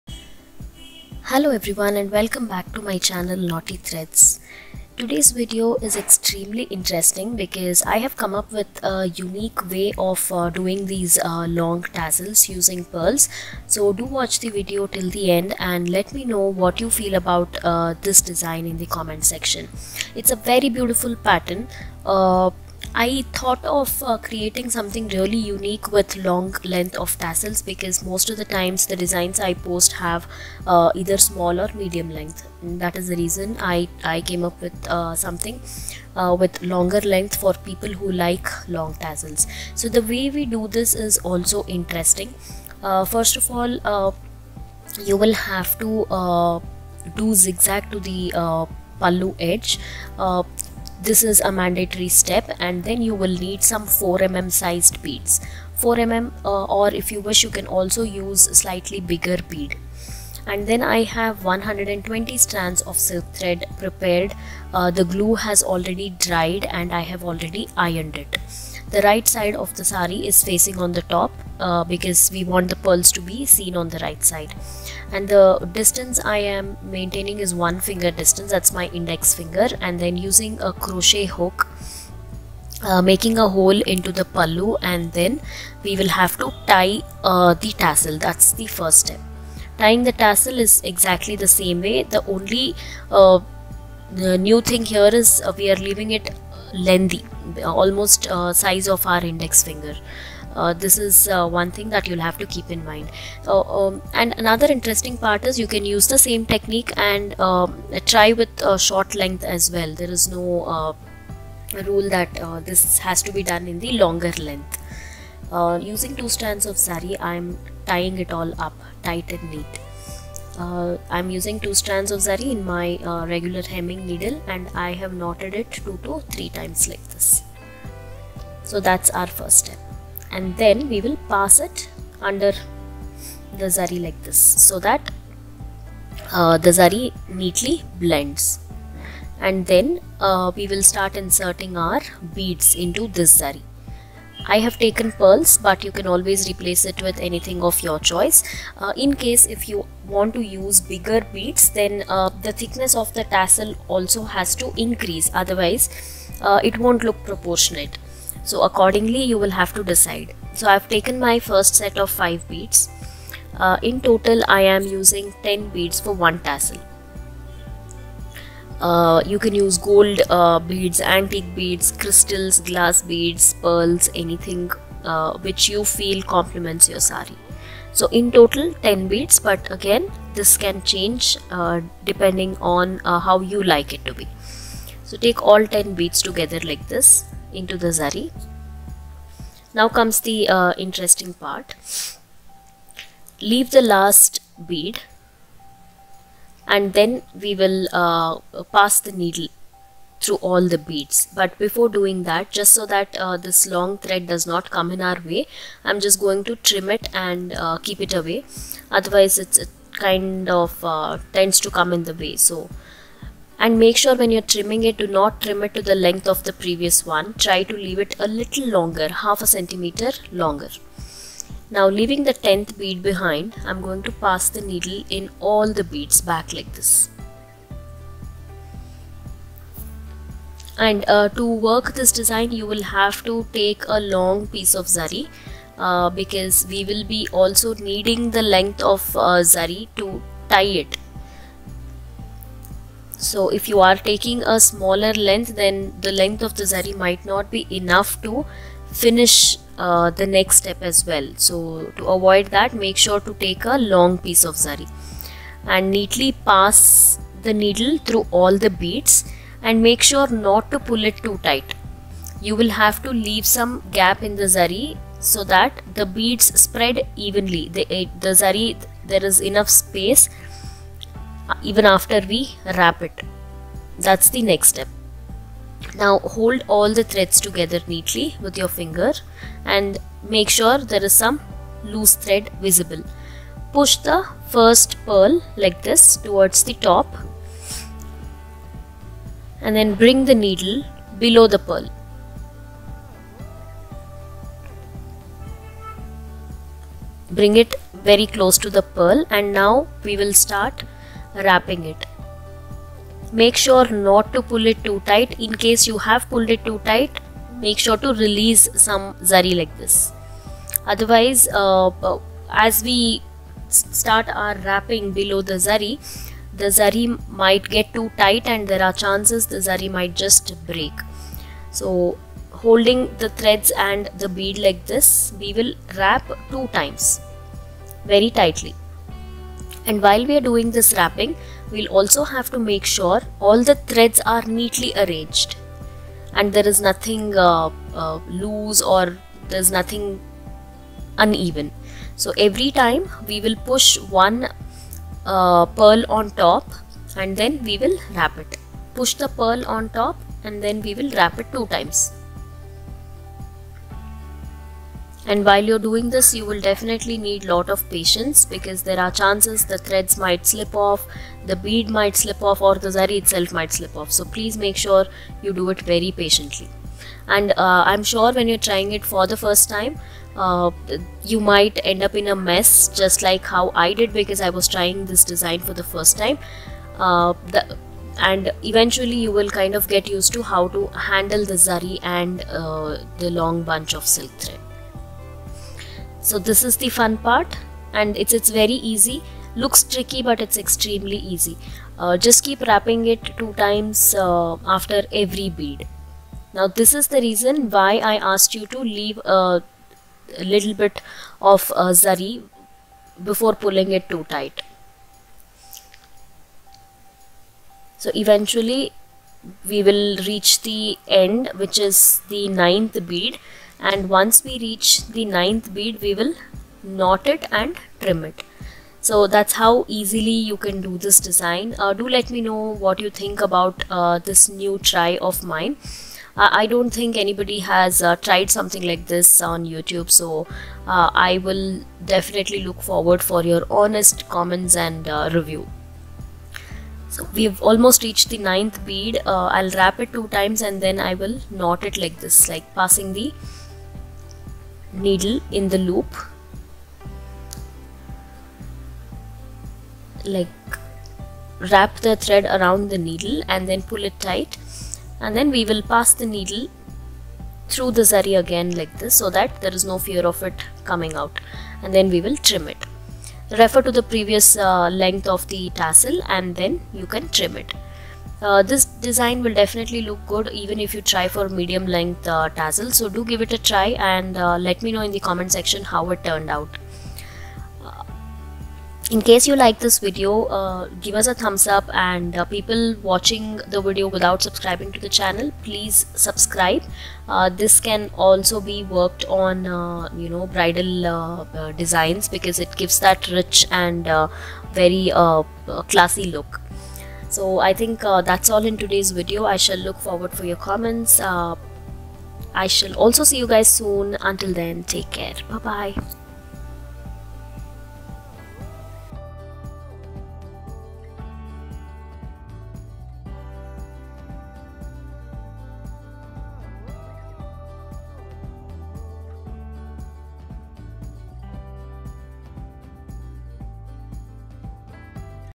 Unique Saree Kuchu Design Using Normal Needle and Pearls - FIRST TIME EVER on YOUTUBE
Easy & Quick Saree Kuchu Design Using Normal Needle - Very Easy For Beginners sareekuchu tassels | Knotty Threadz By Kanika Reddy

Take Care ... See u soon ...

Happy Crafting !!!

♥ ♥ ♥ ♥  Spread Love  ♥ ♥ ♥ ♥